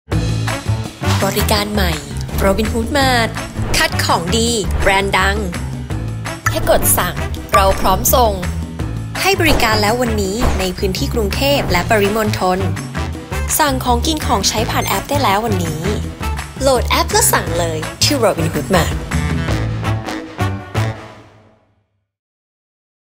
Robinhood Mart
​Robinhood Mart บริการใหม่ล่าสุดจากแอป Robinhood เปิดตัวอย่างเป็นทางการแล้ววันนี้
ให้คุณสะดวกสบายยิ่งขึ้น ไม่ต้องออกไปซื้อเอง เพียงสั่งซื้อสินค้าผ่านแอป ทั้งของสด ของกินของใช้ และสินค้าอื่น ๆ มากมายจากหลากหลายร้านค้าและซุปเปอร์มาร์เก็ตชั้นนำ พร้อมโปรโมชันสินค้าราคาพิเศษและโค้ดส่วนลดร้านค้าอีกเพียบ

​Robinhood Mart คัดของดี แบรนด์ดัง คุณสั่ง เราพร้อมส่ง

一
*เงื่อนไขเป็นไปตามบริษัทฯ กำหนด
โหลดแอปแล้ว สั่งเลย!